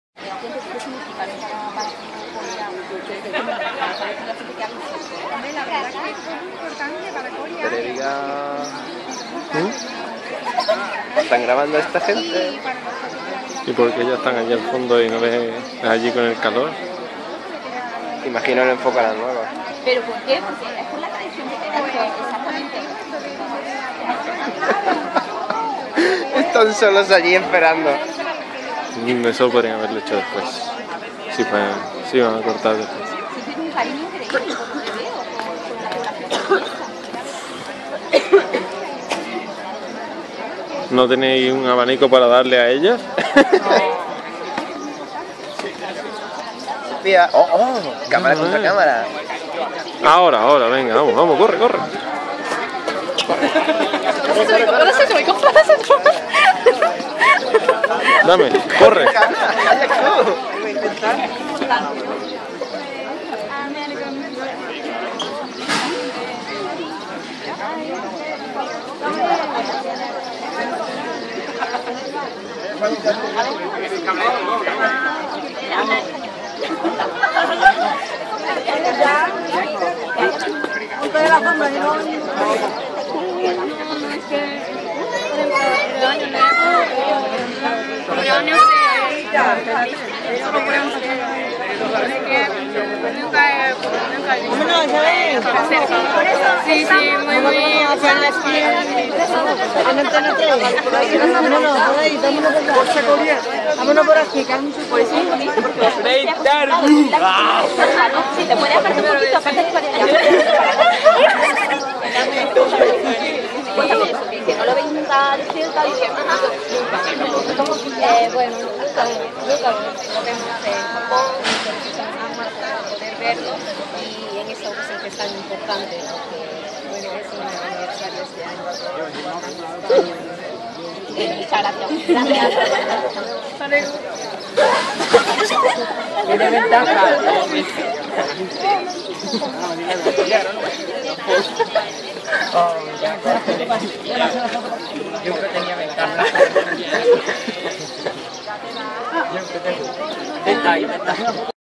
Mio, Natsuki, Miyako, Kana y Sofía en Antena 3!
El quinteto japonés saliendo por la Televisión española en la visita del príncipe Naruhito a Coria del Río